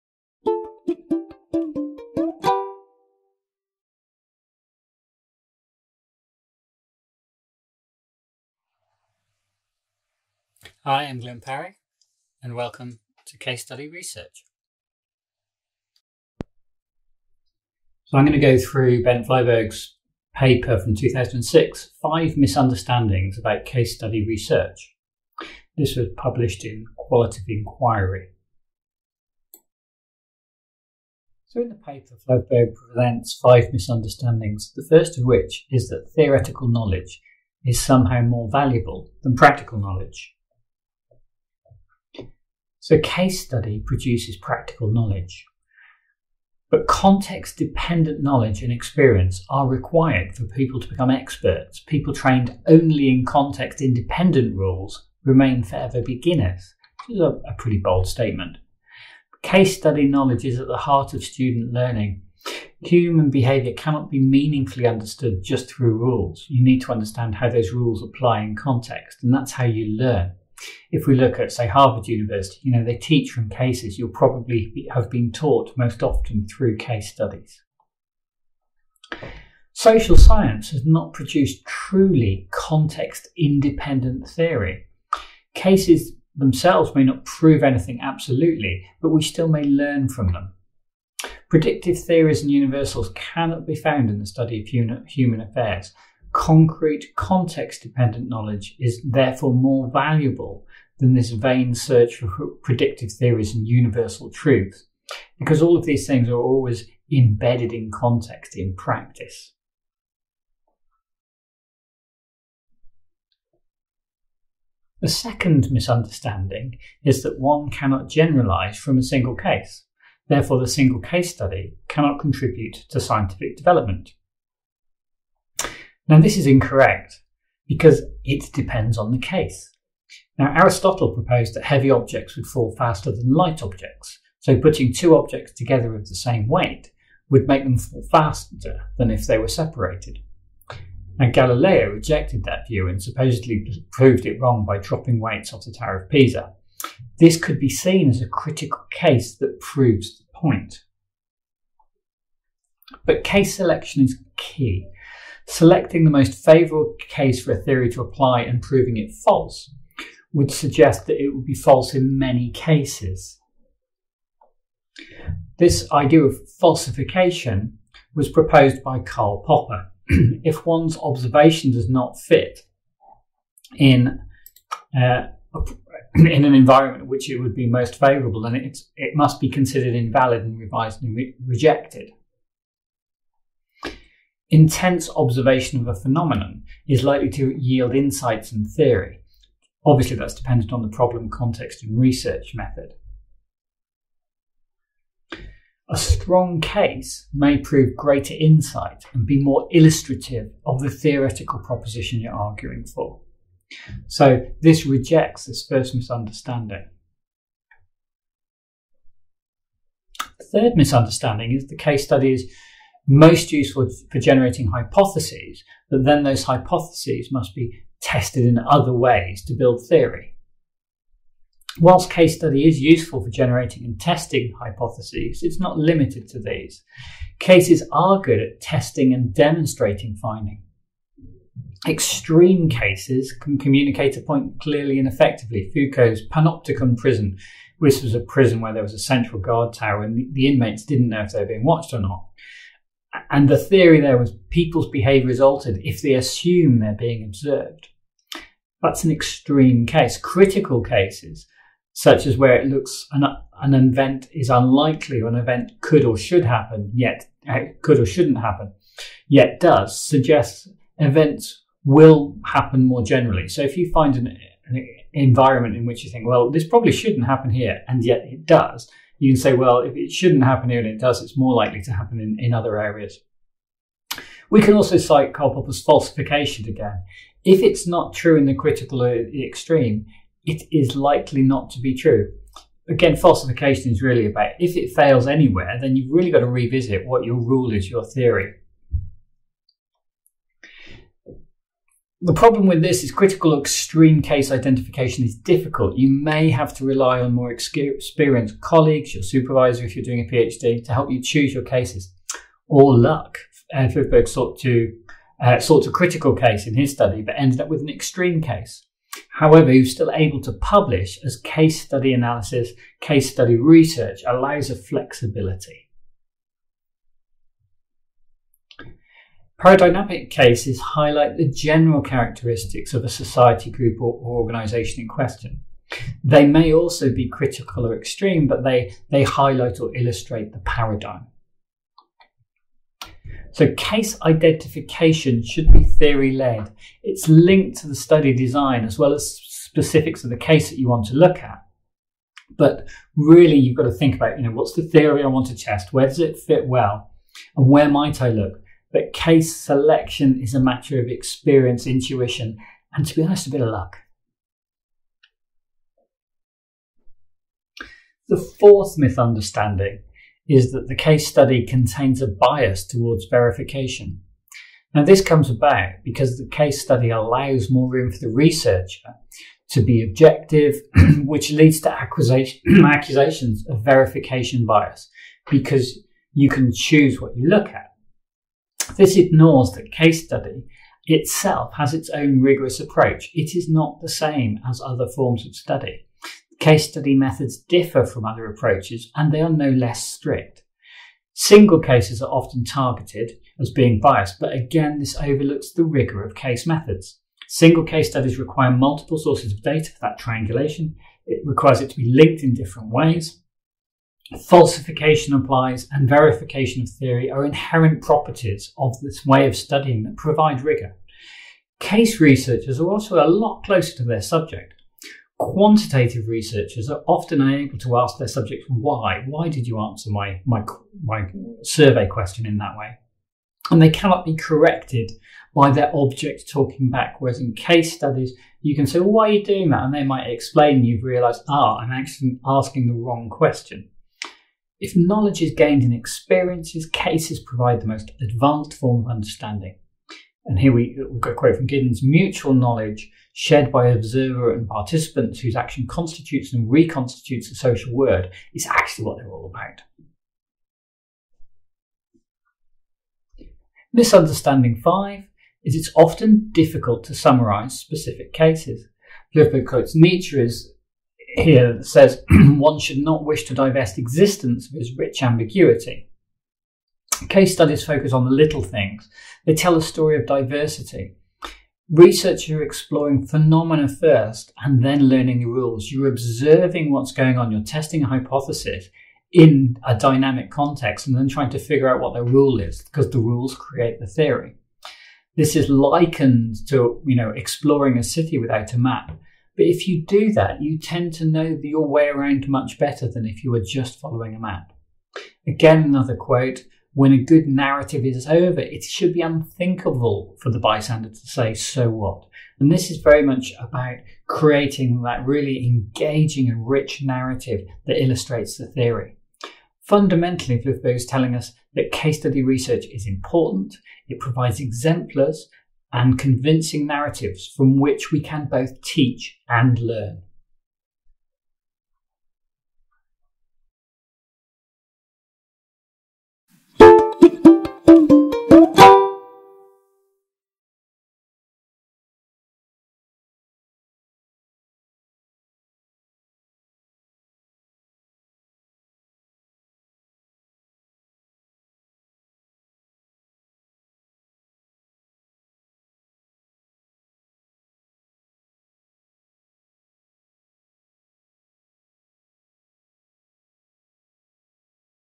Prof Parry describes the paper "Five Misunderstandings About Case-Study Research" by Flyvbjerg 2006
Our paper explanation series continues with Prof Glenn Parry describing the paper by Bent Flyvbjerg (2006) “Five Misunderstandings About Case-Study Research” Qualitative Inquiry (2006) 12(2)pp. 219-245